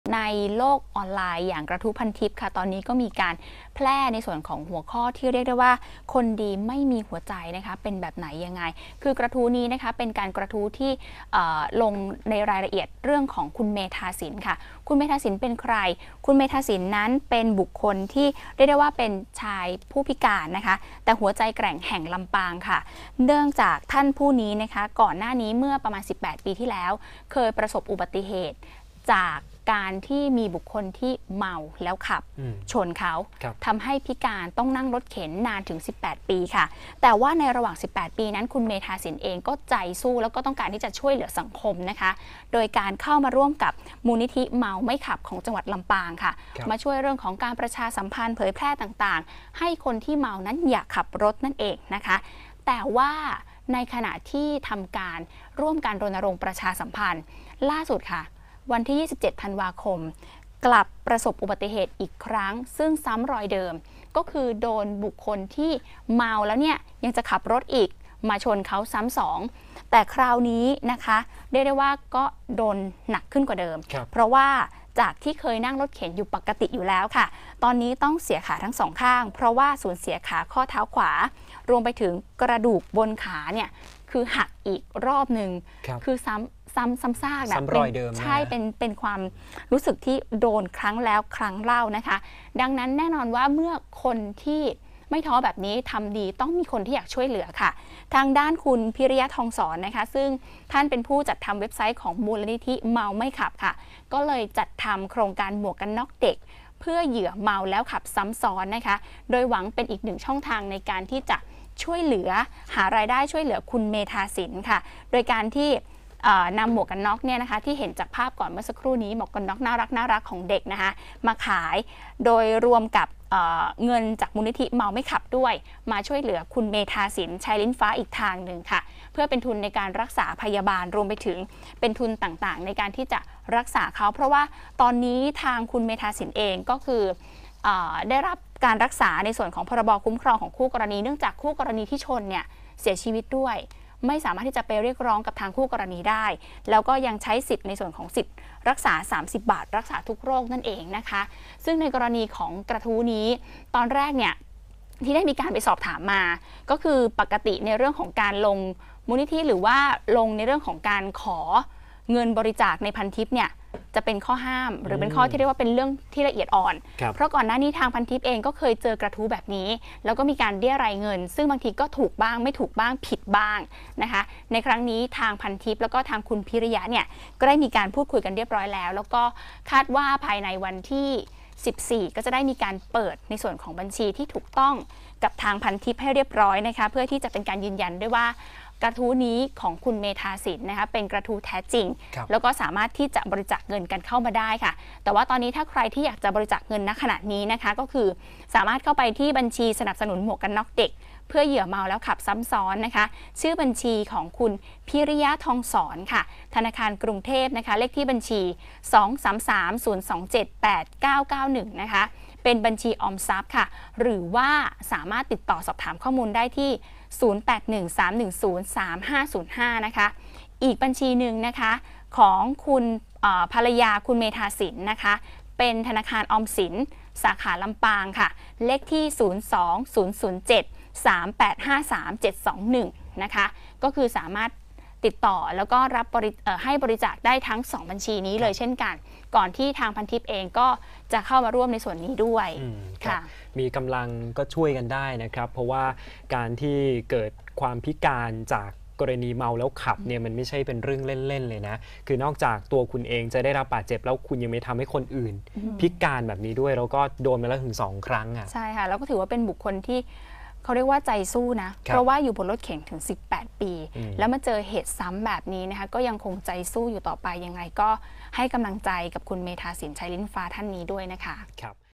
สังคมไทย น้ำใจคนดี เมธาสินธุ์ ชายผู้ต่อสู้
ขณะนี้ในสังคมออนไลน์มีเผยแพร่กระทู้ดังในเว็บไซต์พันทิปดอทคอม ที่มีผู้ใช้นามแฝงว่า คนดีไม่มีหัวใจ ได้มาตั้งกระทู้ไว้ เพื่อเป็นการช่วยเหลือ คุณเมธาสินธุ์ ชายผู้พิการ หัวใจแกร่งแห่งลำปาง โดยมีผู้ที่เข้ามาแสดงความคิดเห็นเป็นจำนวนมาก